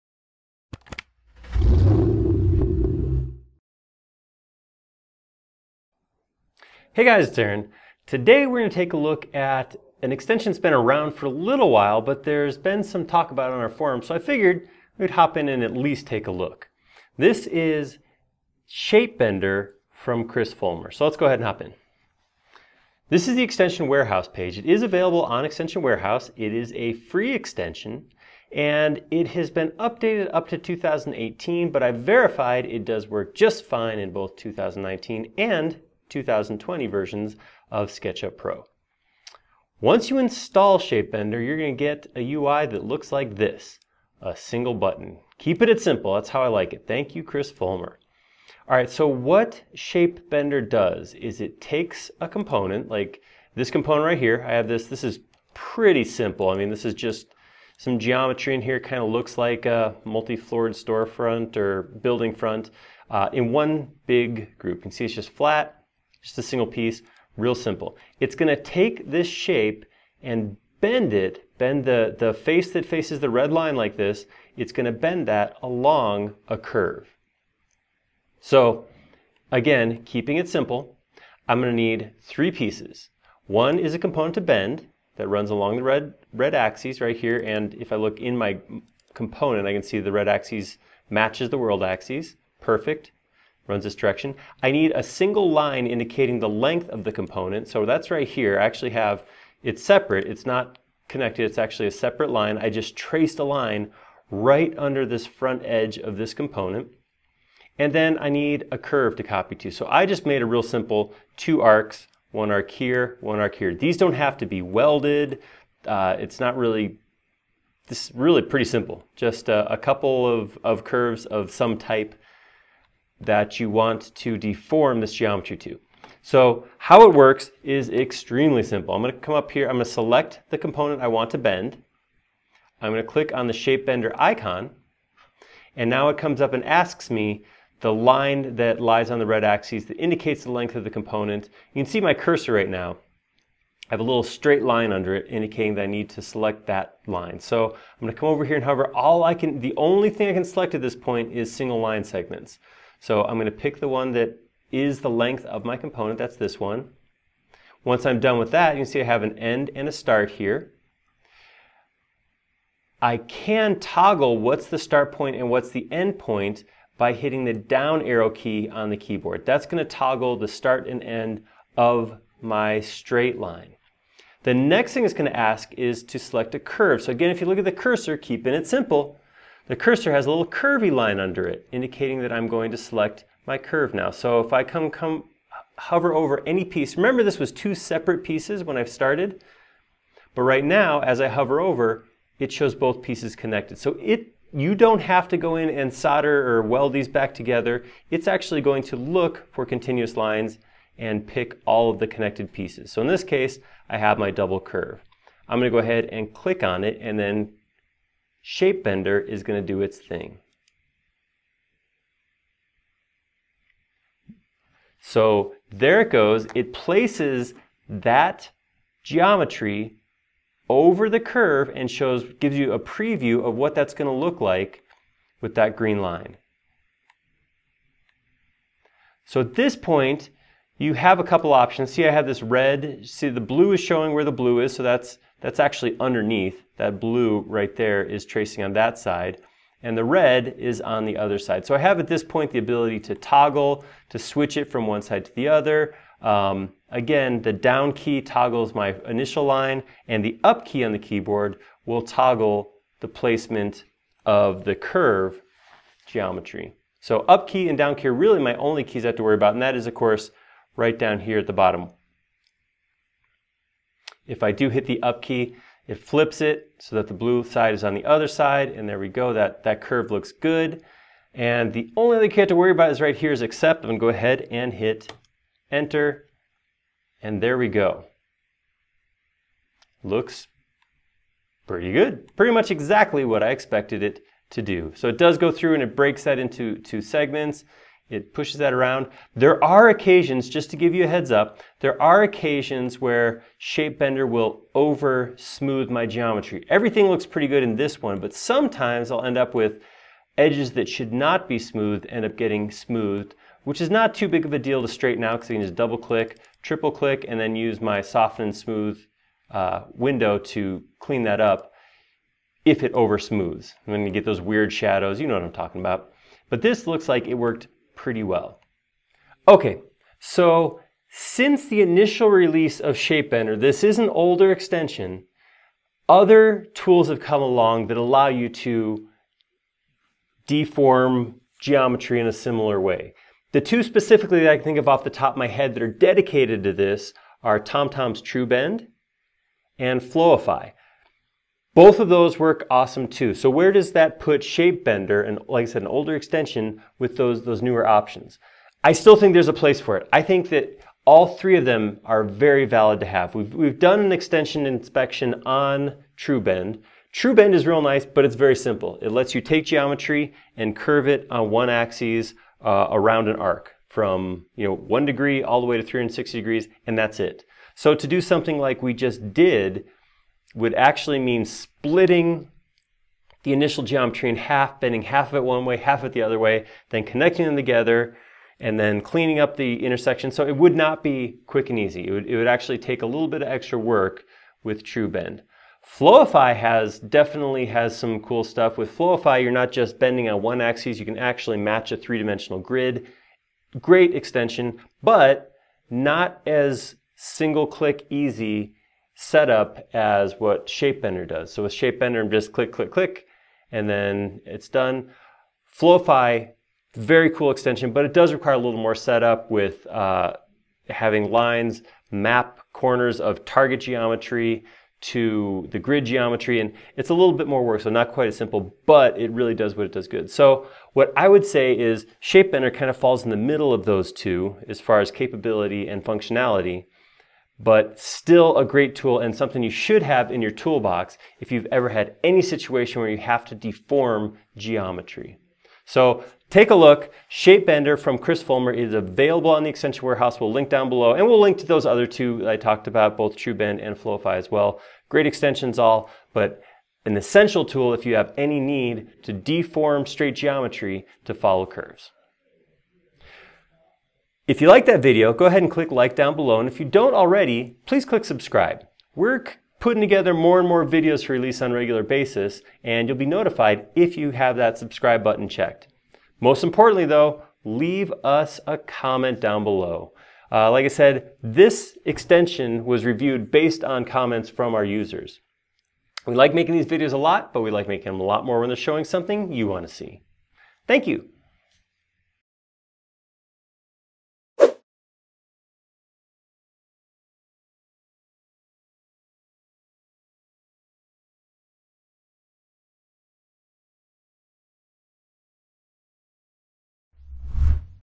CFL Shape Bender - Extension Inspector
We're taking a look at an extension that has been around for a little while: Shape Bender from Chris Fullmer. This extension is free on Extension Warehouse. Follow along as we show you how Shape Bender can take a component and bend it along a curve.